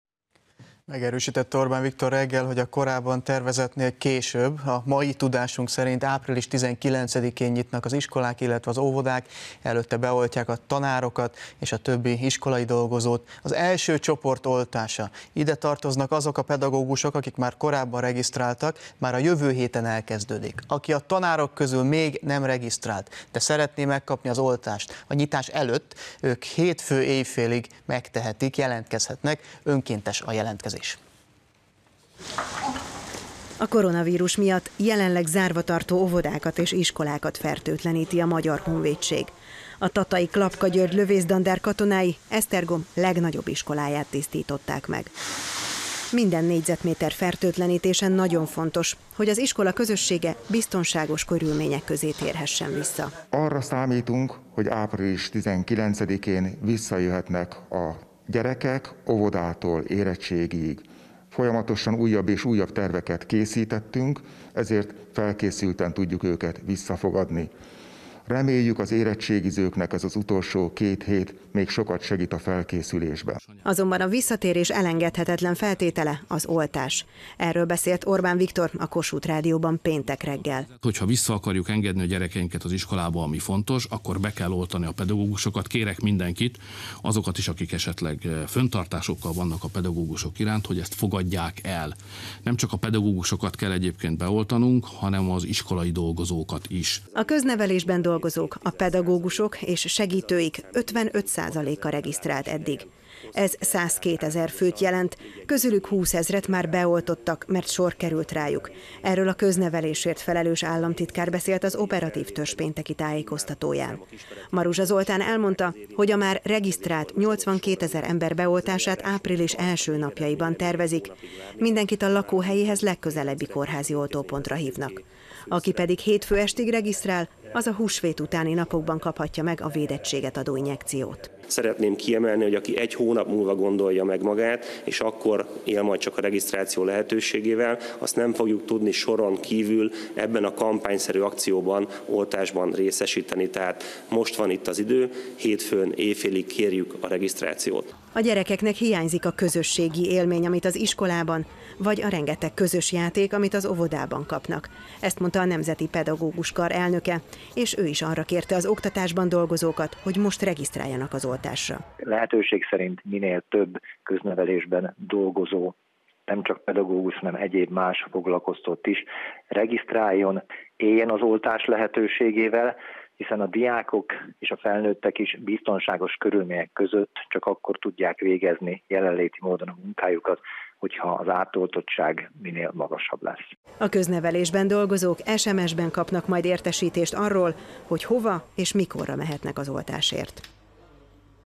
Elkezdődik a pedagógusok oltása
Megerősítette Orbán Viktor azt is reggel, hogy a korábban tervezettnél később, a mai tudásunk szerint április 19-én nyitnak az iskolák és az óvodák. Előtte beoltják a tanárokat és a többi iskolai dolgozót. Az első csoport oltása - ide tartoznak azok a pedagógusok, akik már korábban regisztráltak - már a jövő héten elkezdődik. Aki a tanárok közül még nem regisztrált, de szeretné ha beoltanák a nyitás előtt, hétfő éjfélig megteheti. Önkéntes a jelentkezés.